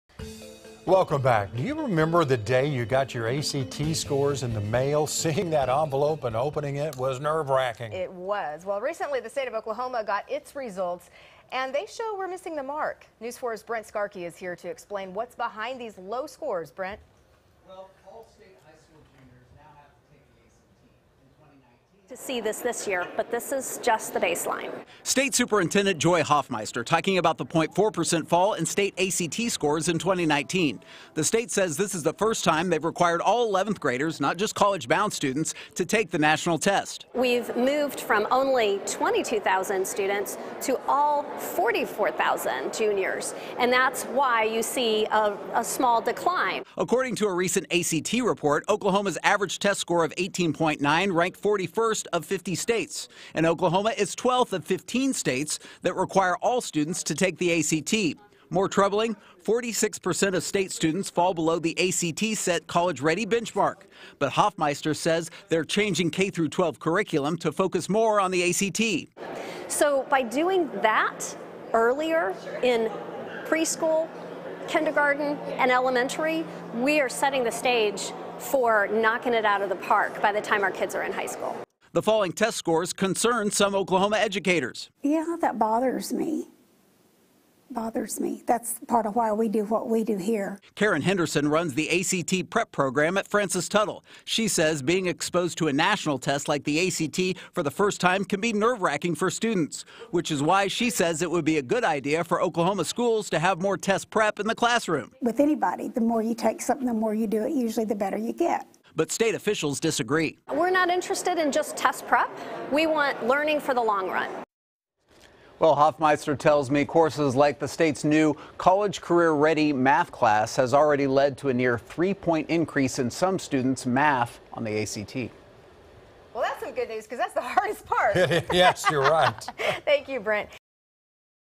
State ACT scores fall
All Oklahoma high school juniors now have to take the ACT.. In 2019, the average ACT score fell in Oklahoma, and almost half of Oklahoma 11th graders failed to qualify as college ready by ACT standards.